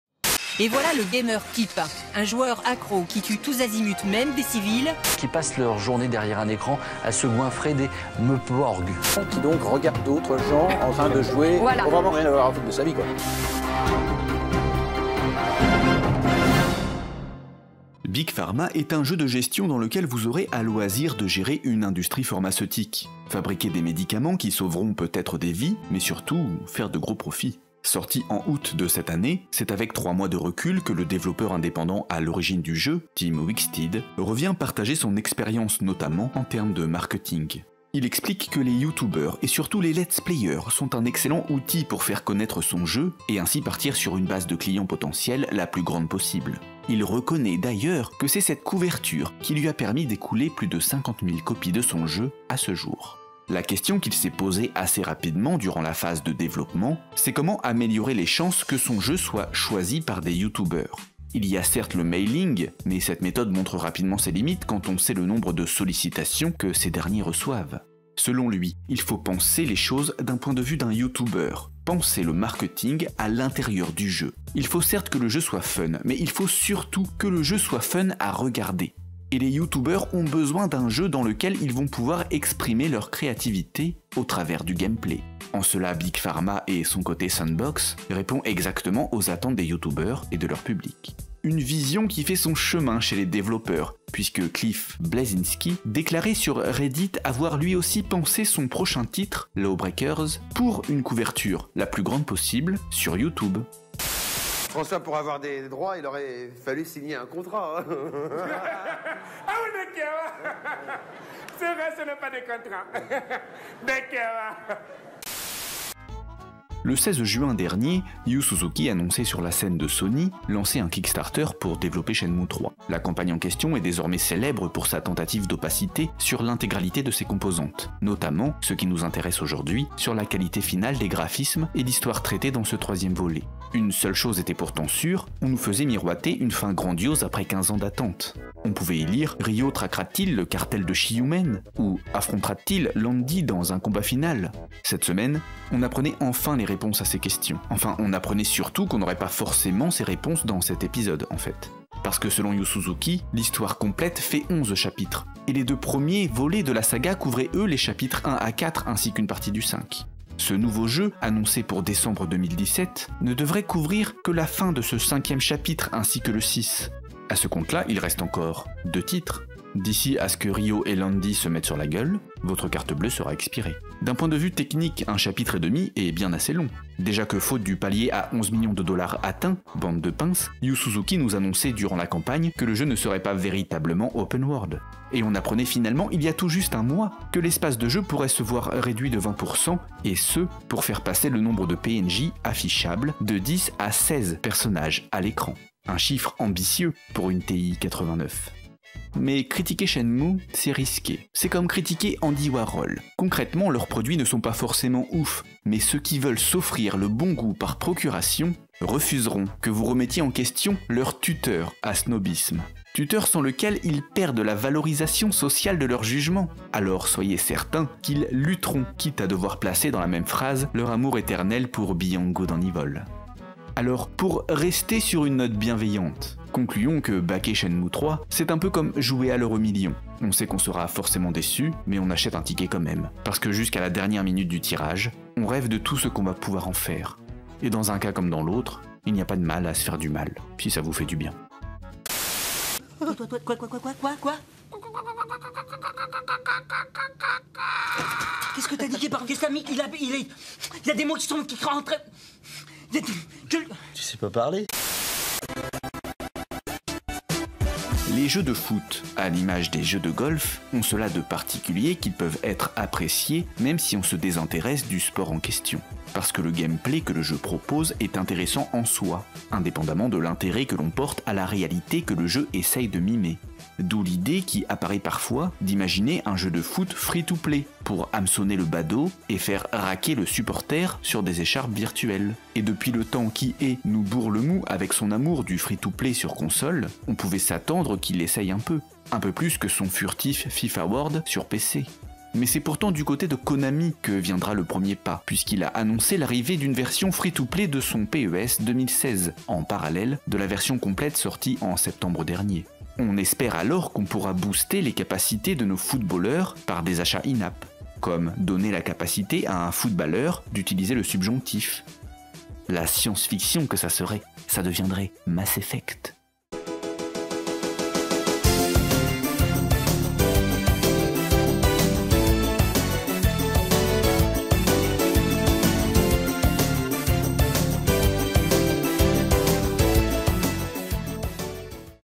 Free2Pay #54 : Youtubeur Ready, Loto Shenmue 3 & Free to PES
Une semaine après avoir parlé de la perception de la violence dans les jeux, d’un nouveau concurrent pour Hearthstone et de l’opportunisme financier du géant Electronic Arts, l’équipe d’UnDropDansLaMare est de retour avec l’épisode 54 de Free2pay.

Et en cette période d’hiver où les maladies sont là pour nous pourrir la vie, à grands renforts de toux, fièvres et autres états patraques, quoi de mieux qu’un petit coup d’œil à Big Pharma et à son développeur indépendant qui, pour s’assurer du succès de son jeu, a dû le penser comme un produit marketing d’utilité pour les Youtubeurs... Après, il sera temps de se pencher sur une autre maladie, puisque ce sera l’occasion, avec Shenmue 3, de voir à quel point un produit, dont on ne connaît rien, peut à ce point retourner le cœur des fans à travers le monde, malgré son opacité... Et en dernier lieu, l’heure sera à se moquer un brin des footballeurs et de l’annonce du passage en free to play de PES et de son utilisation du subjonctif...